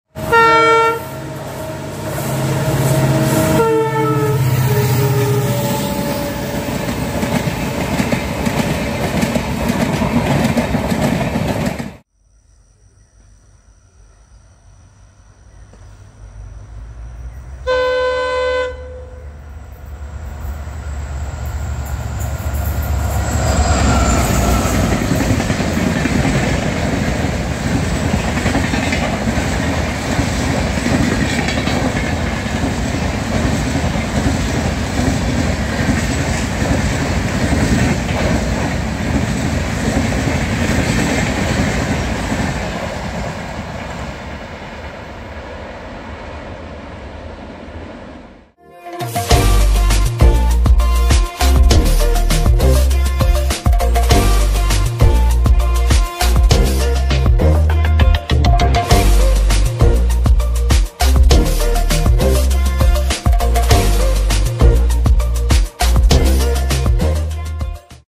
22634 Hazrat Nizamuddin - Trivandrum Cntl SF express (Konkan railway)
22633 / 22634 Thiruvananthapuram Central–Hazrat Nizamuddin Express is a Express train belonging to Indian Railways – Southern Railway zone that runs between Thiruvananthapuram Central and Hazrat Nizamuddin in India.

Train covers total distance of 2845Kms in 45hrs-20mins with Average speed of 62KMPH and Maximum permissible speed of 130KMPH (Vasai road - Nizamuddin)

It operates as train number 22633 from Thiruvananthapuram Central to Hazrat Nizamuddin and as train number 22634 in the reverse direction, serving the states of Kerala, Karnataka, Goa, Maharashtra, Gujarat, Madhya Pradesh, Rajasthan, Uttar Pradesh, Haryana & Delhi

22633 / 22634 Thiruvananthapuram Central–Hazrat Nizamuddin Express have 1 AC 2 tier, 6 AC 3 tier, 10 Sleeper class 3 tier, 4 General Unreserved & 2 EOG (Seating cum Luggage Rake) coaches. It does not carry a pantry car.[2]

As it is customary with most train services in India, coach composition may be amended at the discretion of Indian Railways depending on demand

Both trains are hauled by a Ernakulam-based WDP-4D diesel locomotive from Thiruvananthapuram to Vadodara, after Vadodara both trains are hauled by a Vadodara-based WAP-7 electric locomotive till Hazrat Nizamuddin and vice versa